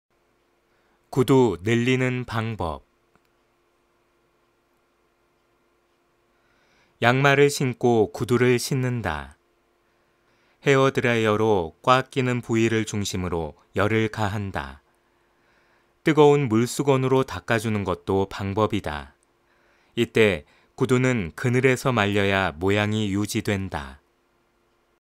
구두 늘리는 방법 (좋은생각 8월호)
양말을 신고 구두를 신는다. 헤어드라이어로 꽉 끼는 부위를 중심으로 열을 가한다. 뜨거운 물수건으로 닦아 주는 것도 방법이다. 이때 구두는 그늘에서 말려야 모양이 유지된다.